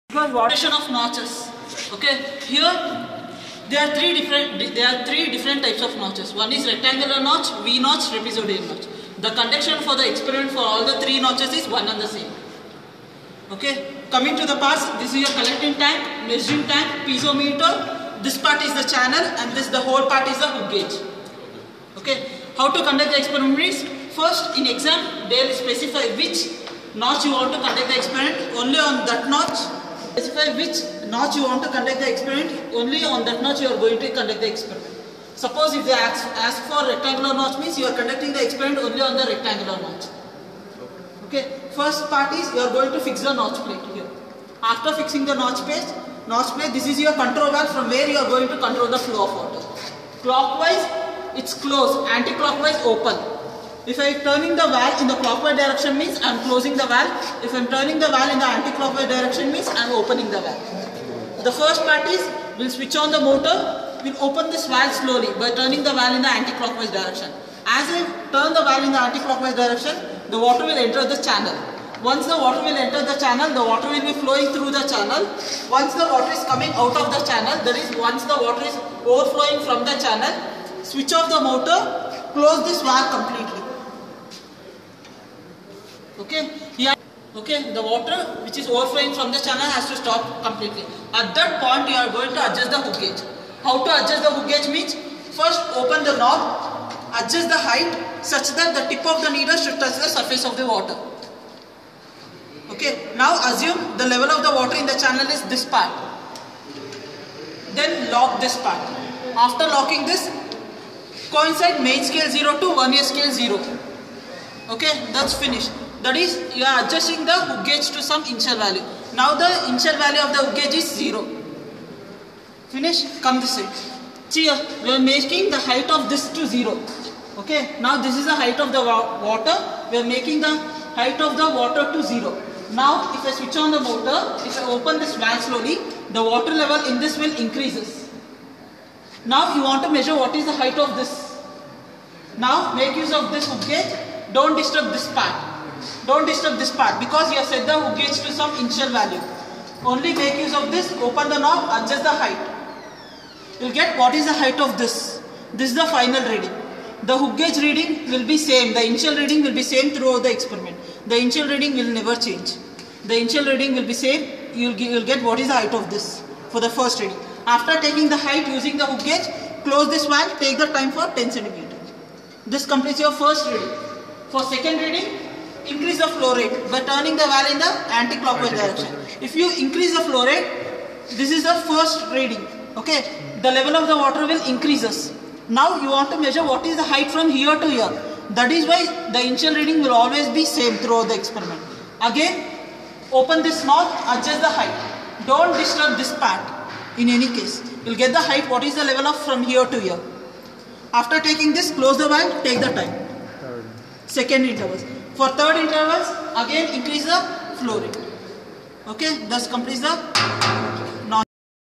FM Lab Experiment Procedure Explanation:To determine coefficient of friction of flow though notches
Fluid Mechanics experiment procedure explanation